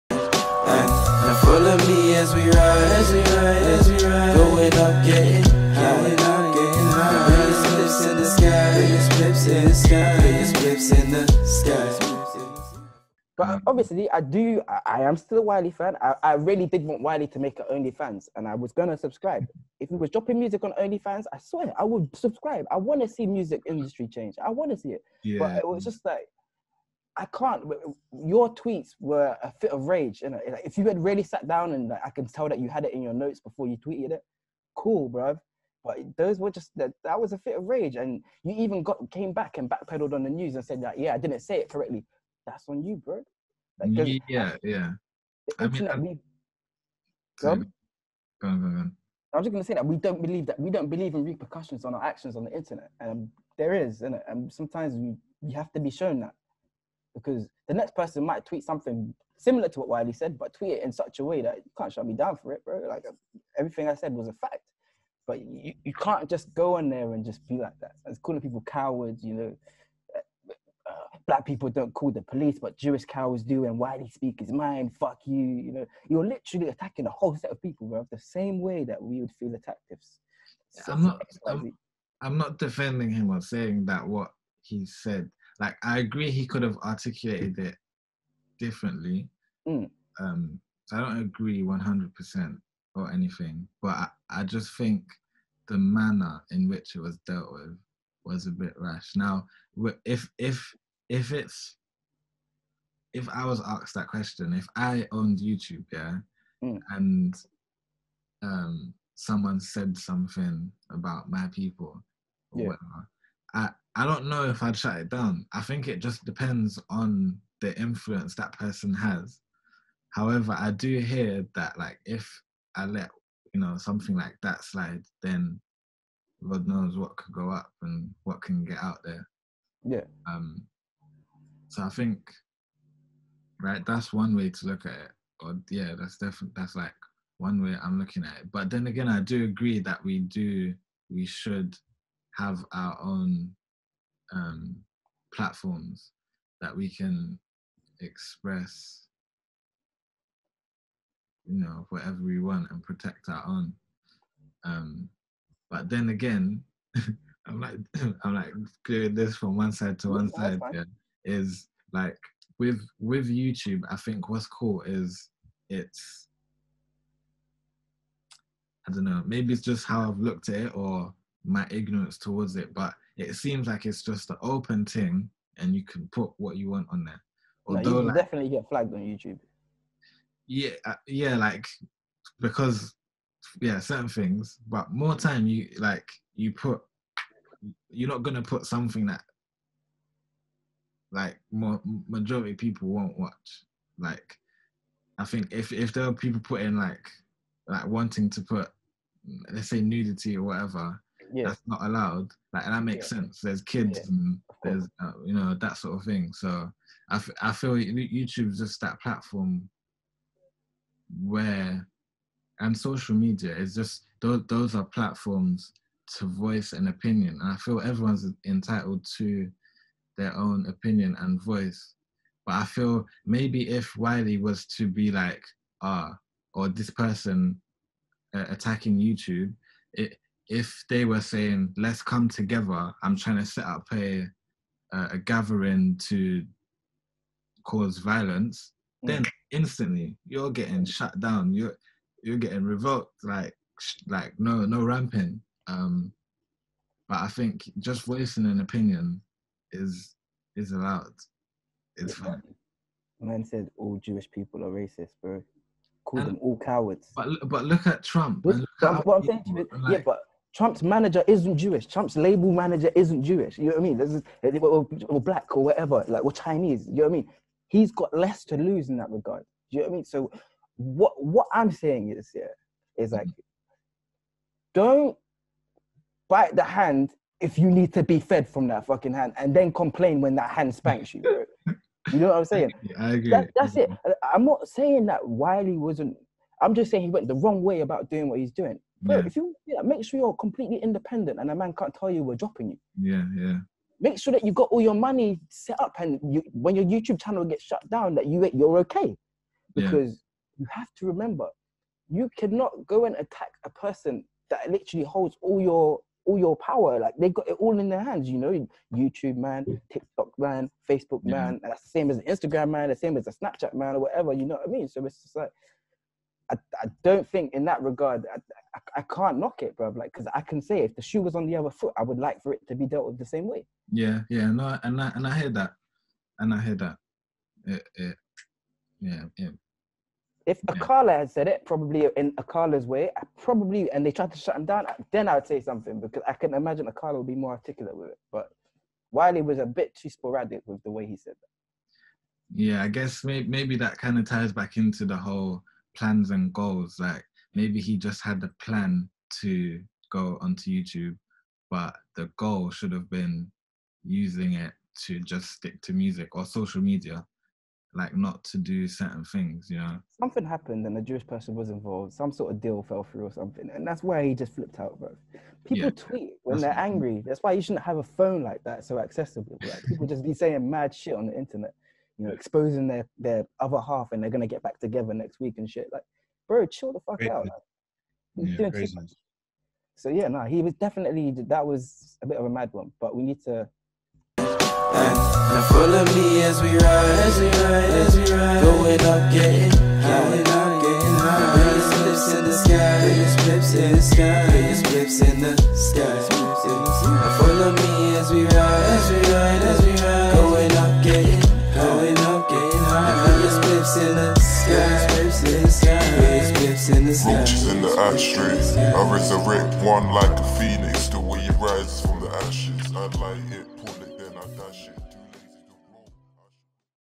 Wiley Treatment Part 2 | The MiVision Show | Episode 59
ThePROGRAMM presents The MiVision Show.
Host Mansa Musa with Special Guest Gbolahan (GB) discuss the treatment Wiley went through in 2020 on the show today.

Let us know what you think of the tune!!

Get with ThePROGRAMM.

Gbolahan (GB) - @gb1k_